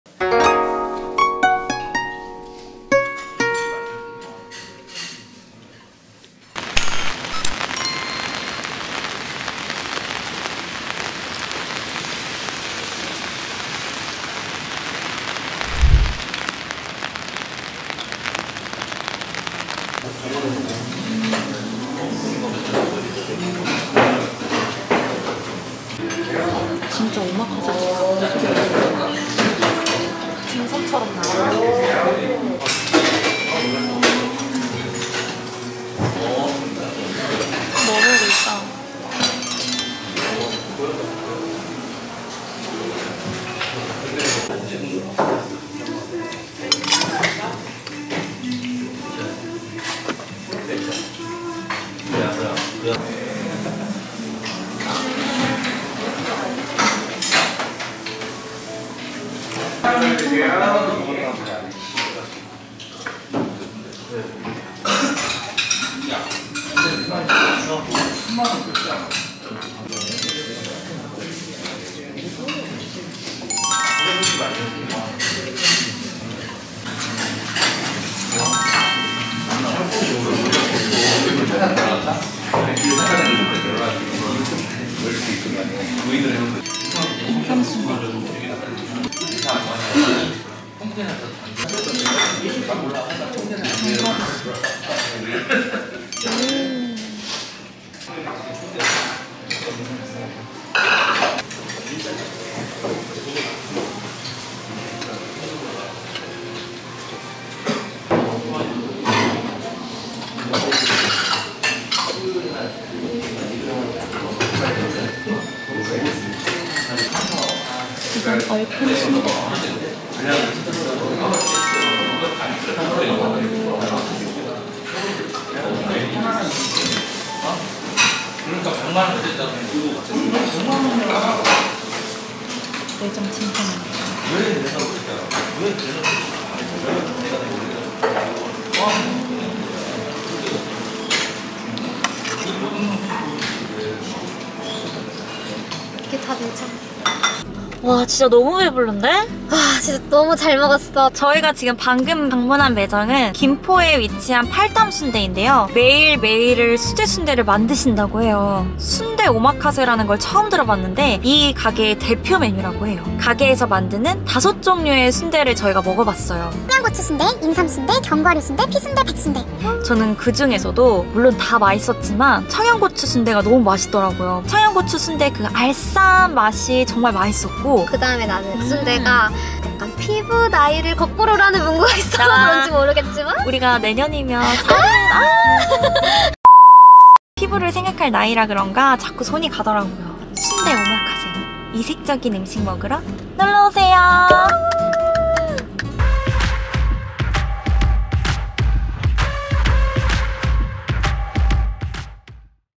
한국인의 소울푸드, 수제 순대 오마카세 맛집
직접 개발한 인삼 순대 외 4가지
다양한 종류의 수제 순대 맛집~!